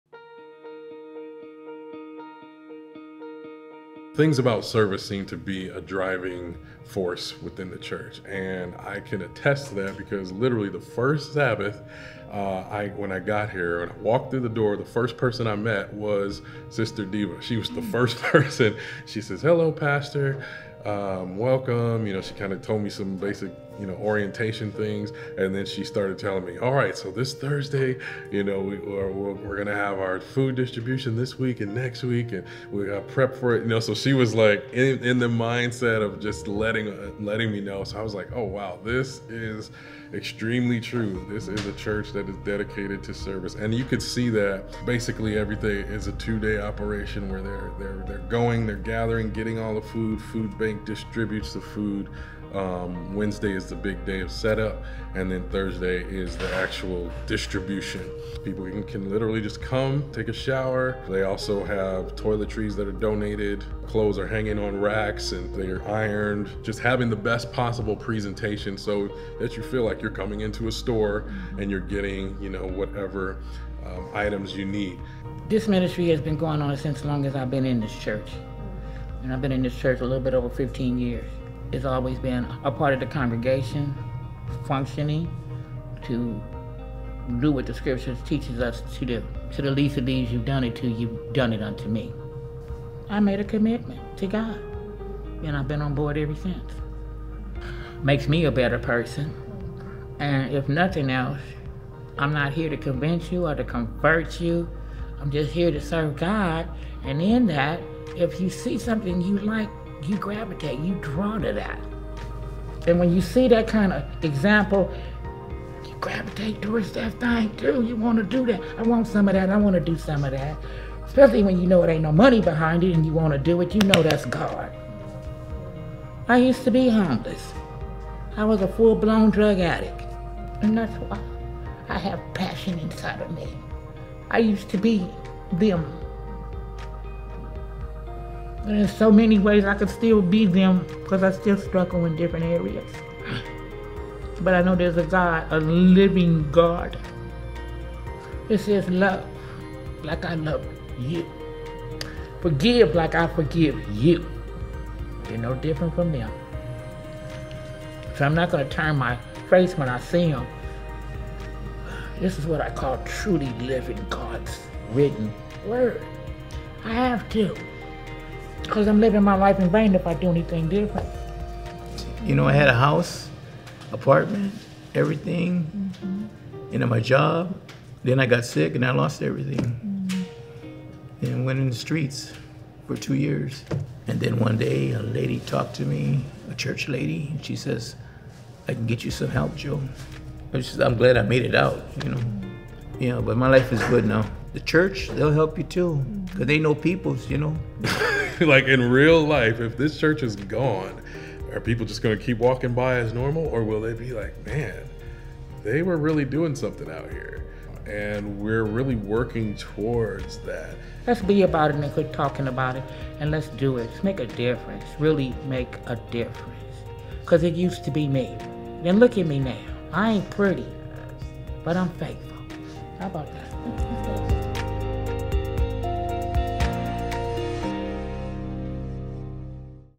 Ephesus: The Serving Church
Ephesus Church talks about their food pantry ministry and how it has changed the lives of the people it serves.